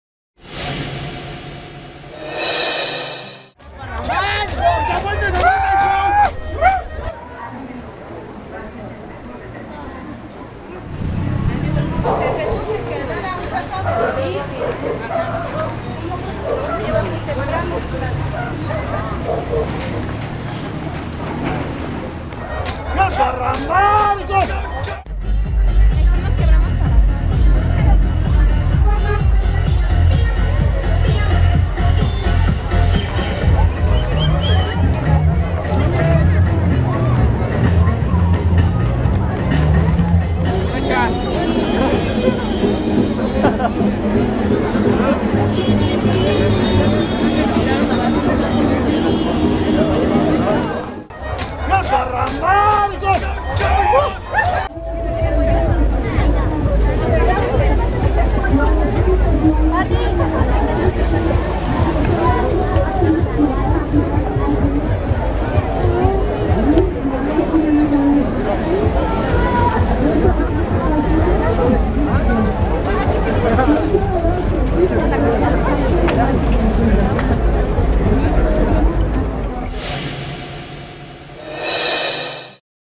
Paseo san Isidro Labrador, Metepec2013 1de 2
Desfile de los locos, en Metepec esta tradicion viene cada año a la festividad de San Isidro Labrador, Mojigangas, carros alegoricos, paseo san isidro, festividad al patrón de la agricultura.